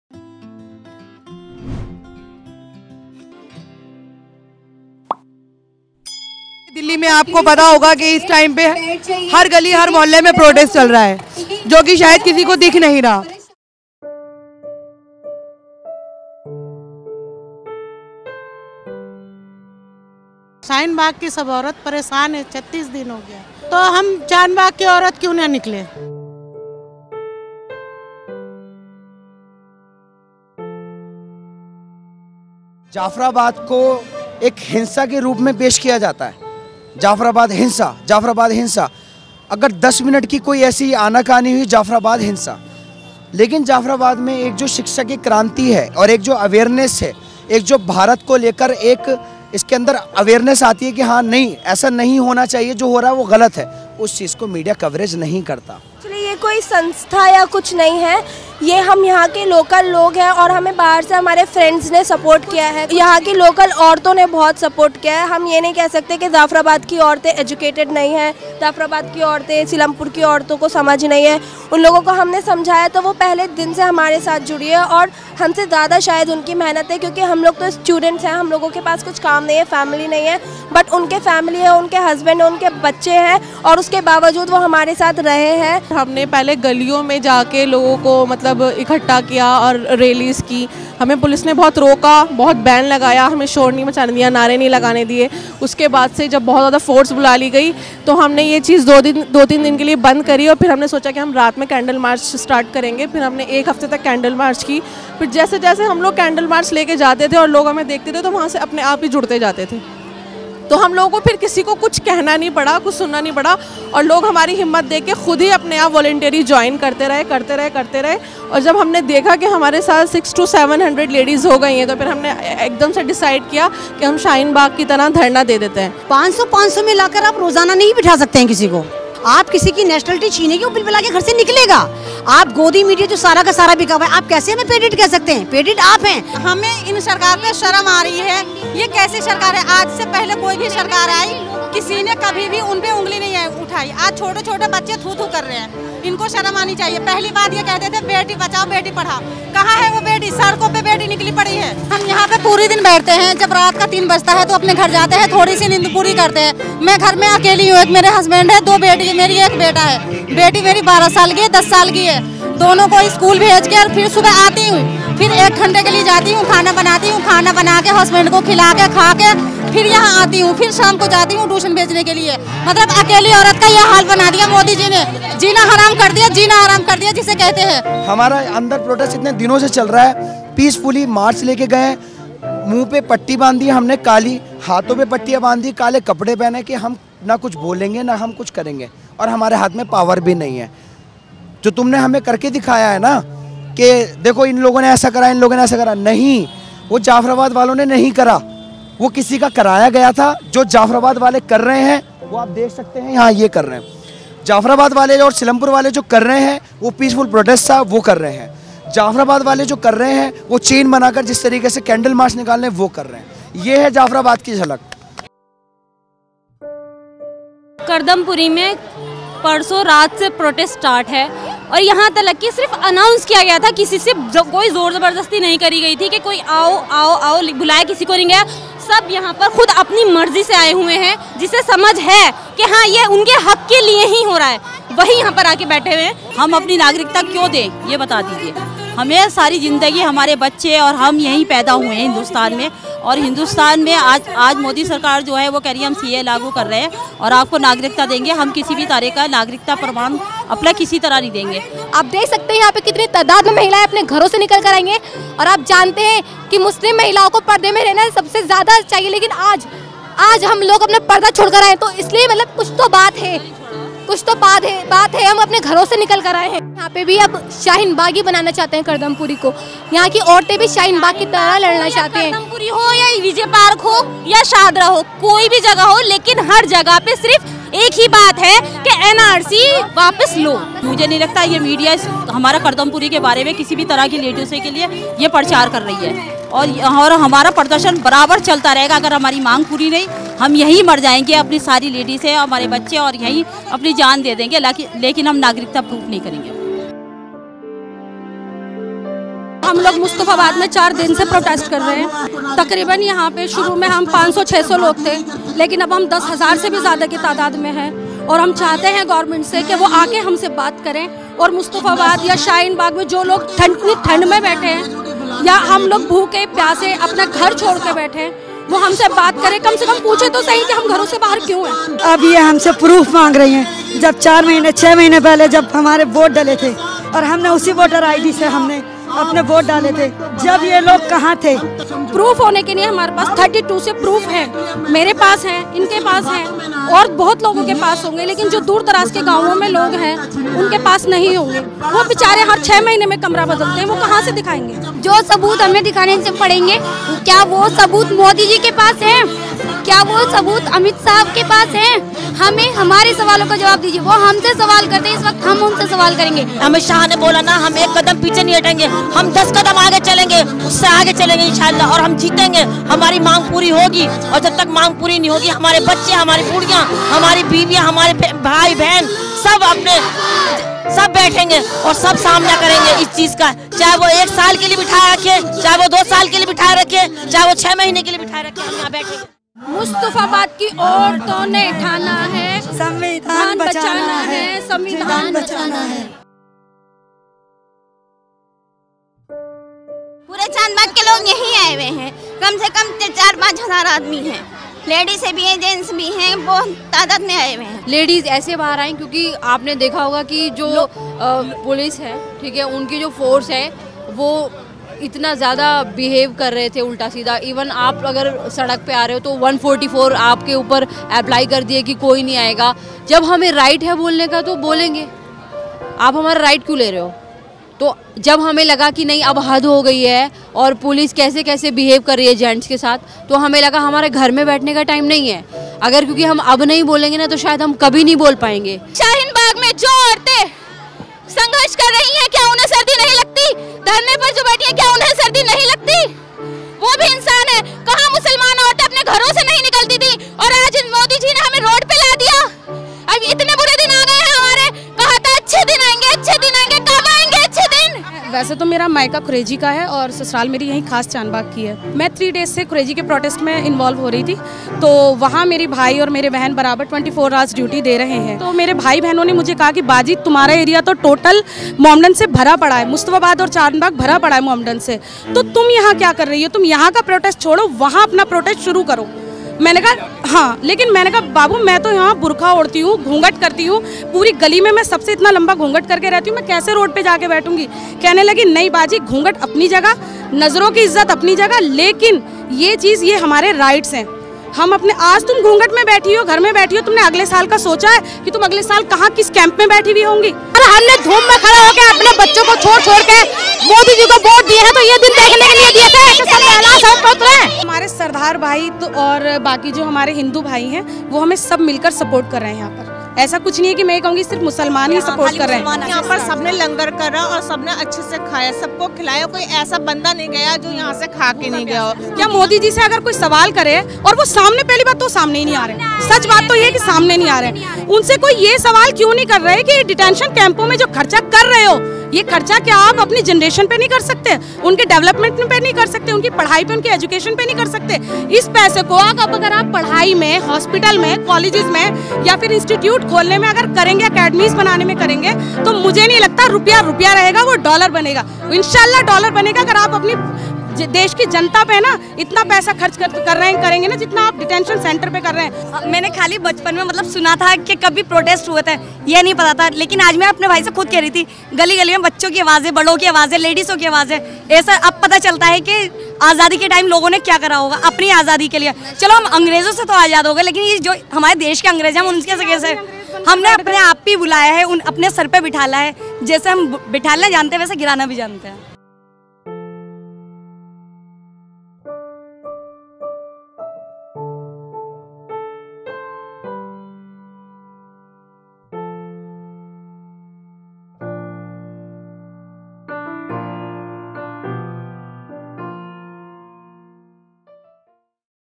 CAA-NRC: इलाका इलाका शाहीन बाग़, चप्पा चप्पा 'आज़ादी'
पूरे देश में विवादित नागरिकता संशोधन अधिनियम और संभावित एनआरसी के ख़िलाफ़ बड़े पैमाने पर विरोध प्रदर्शन हो रहे हैं। इस कड़ी में दिल्ली का शाहीन बाग़ कई आंदोलनों के लिए प्रेरणा बना है। देश में कई शहरों में शाहीन बाग़ बन गए है, या कहें कि शहर शहर शाहीन बाग़ बन रहे हैं। सिर्फ़ दिल्ली की बात करें तो कम से कम 11  इलाकों में विरोध प्रदर्शन शुरू हो गए हैं। शाहीन बाग़ के बाद खुरेजी, जाफ़राबाद, तुर्कमान गेट, भजनपुरा, वजीराबाद , इंद्रलोक , जामिया, खजूरी का श्री राम कालोनी ,कर्दमपुरी और मुस्तफ़ाबाद में भी महिलाओं ने 24*7 धरना शुरू कर दिया हैं। इसके आलावा कई जगह छोटे-बड़े प्रदर्शन चल रहे है।